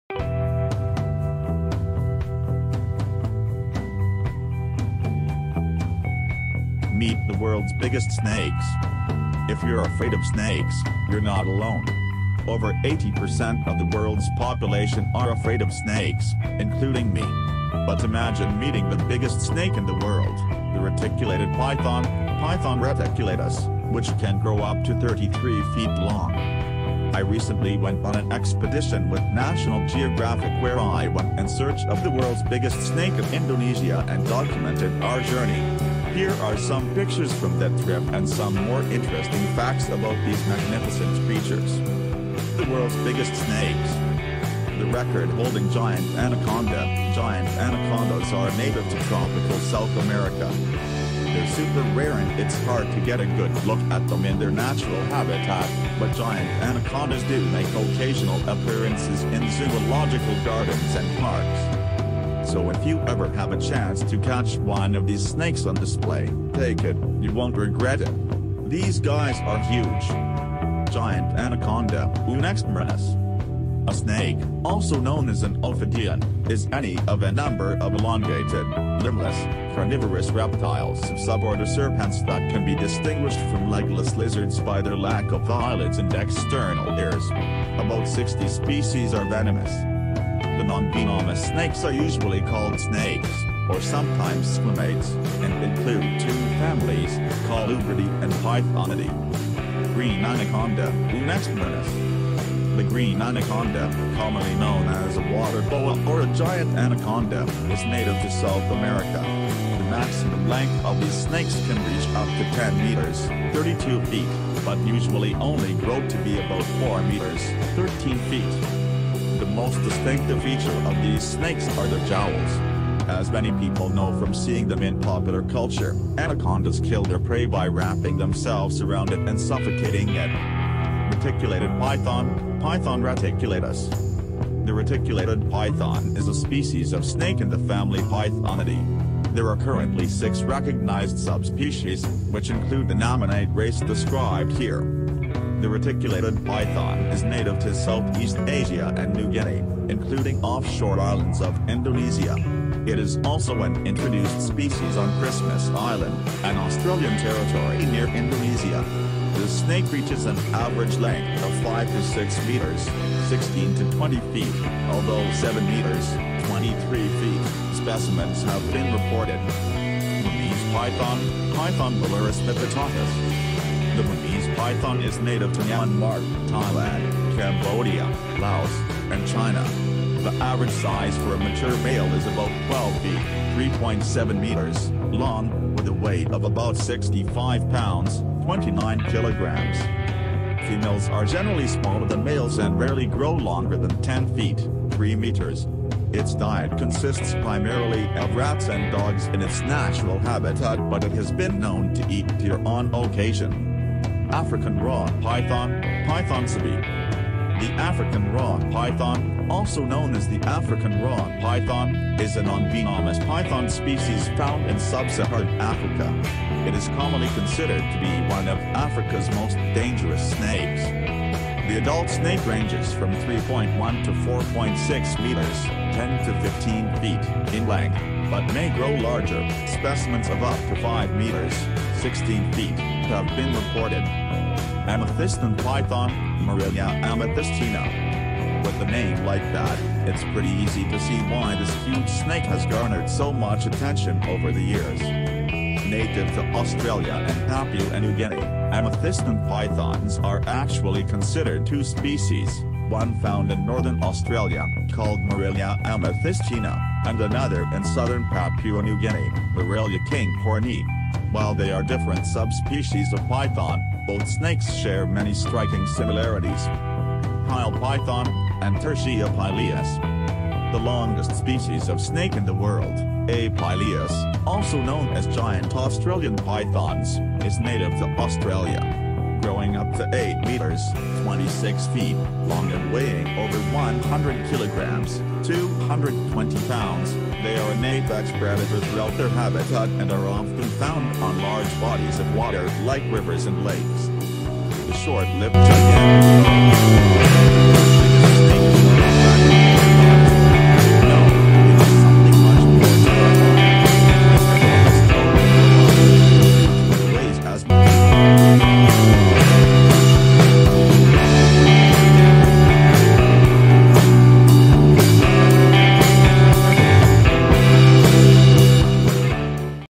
What is the World's Biggest snake
Snakes are known for their ability to swallow big objects, with stories abounding of pythons and anacondas devouring unimaginably large prey. While the subject is filled with myth and exaggeration, most species have anatomical adaptations that make them ideally adapted for managing huge prey. Though most prey weighs a third or less of the snake's body weight, snakes have been known to eat creatures that are much larger than themselves.